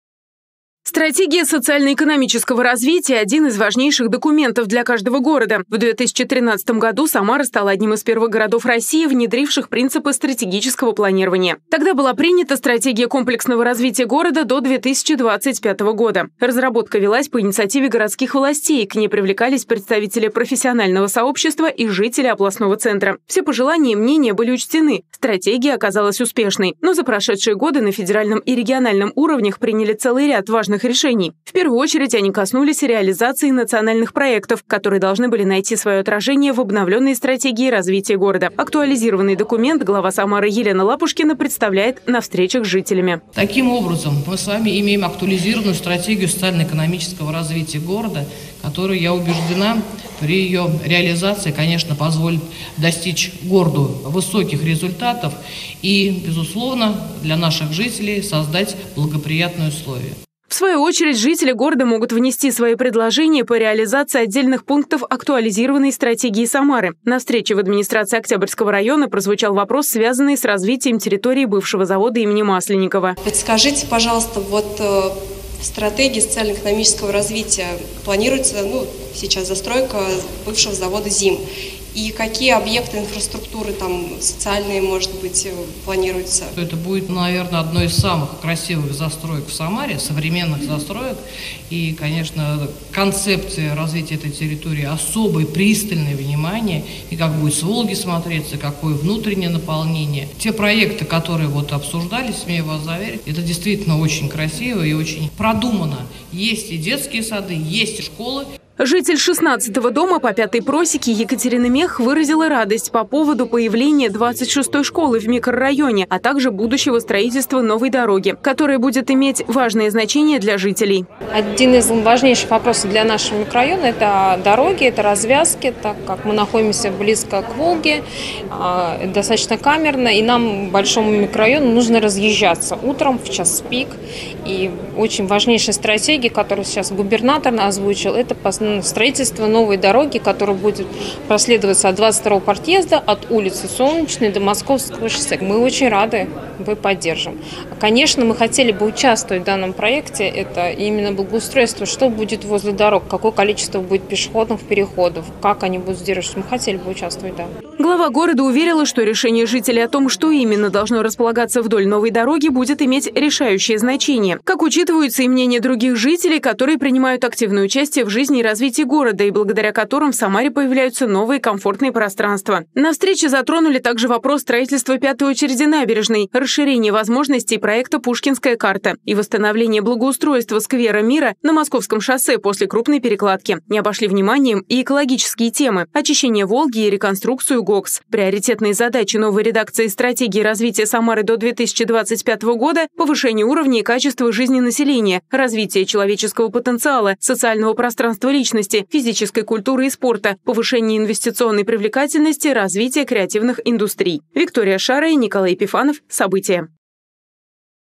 В Самаре обсудили изменения в социально-экономической стратегии города
Глава Самары Елена Лапушкина продолжает представлять обновленную стратегию социально-экономического развития Самары. В этот раз она встретилась с жителями города в администрации Октябрьского района. С какими предложениями к главе обратились жители, в нашем сюжете.